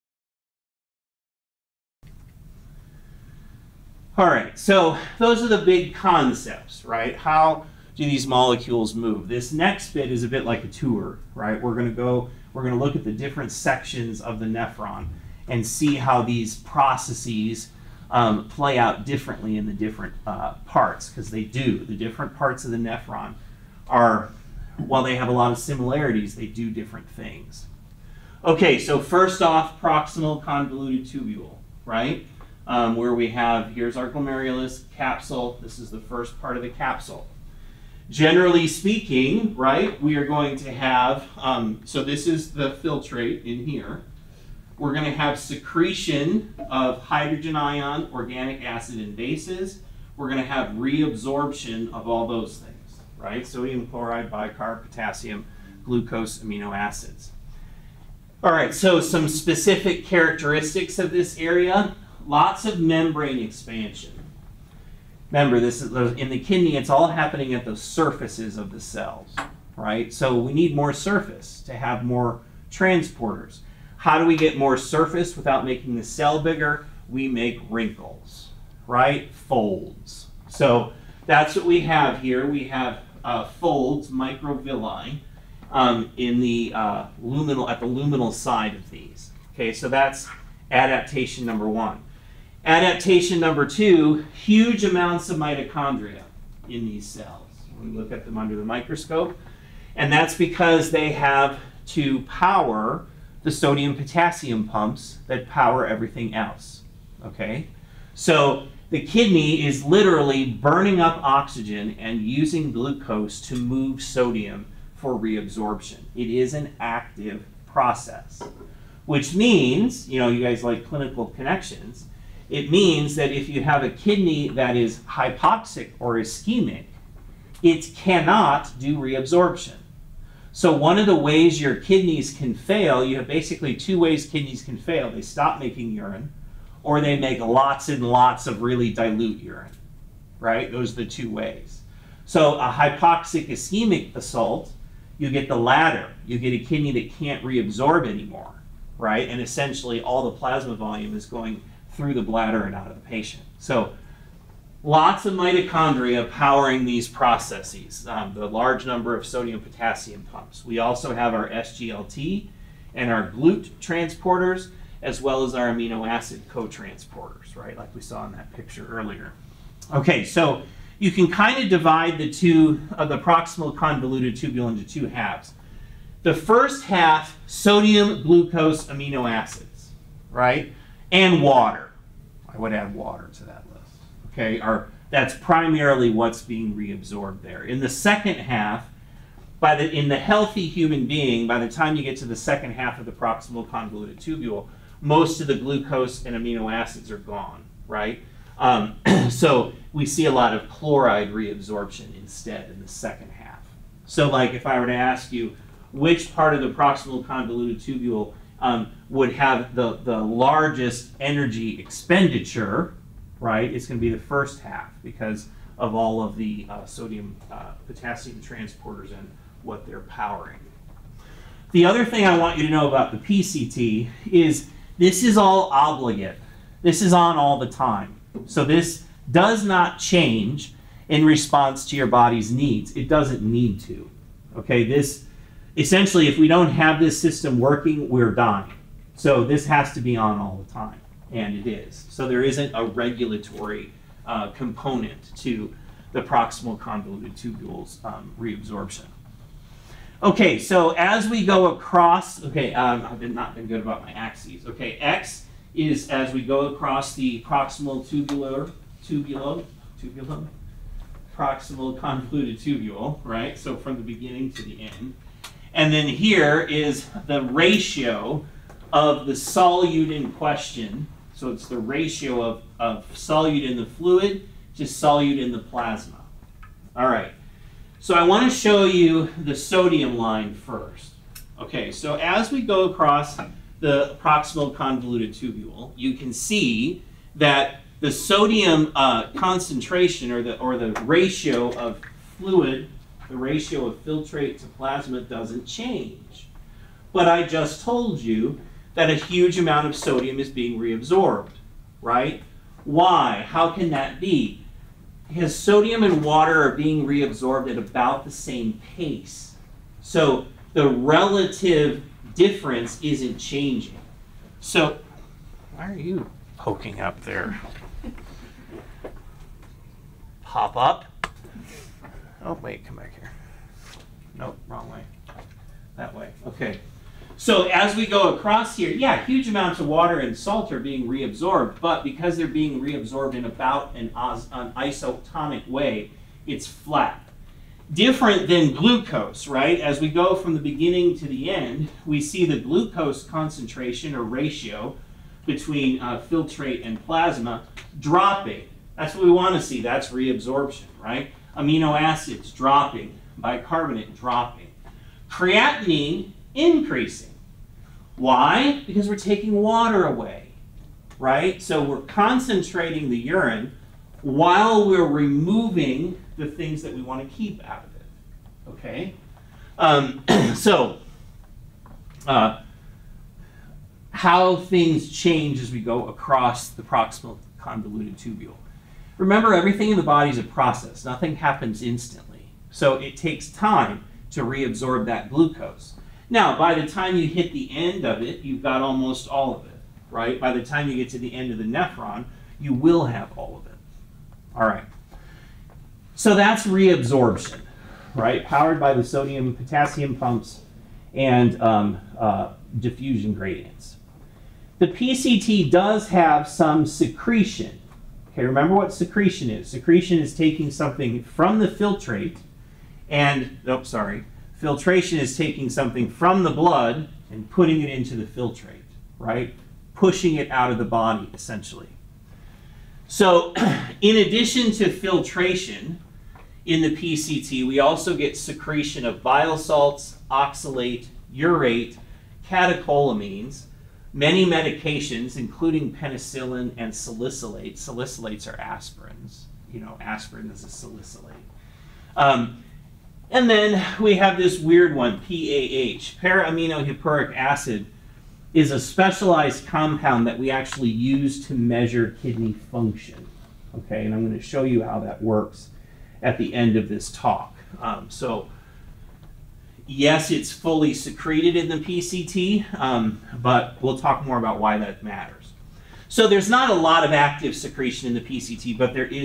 Renal Physiology: Proximal Convoluted Tubule
The proximal convoluted tubule is a major player in obligate reabsorption of ions, glucose, useful molecules and water.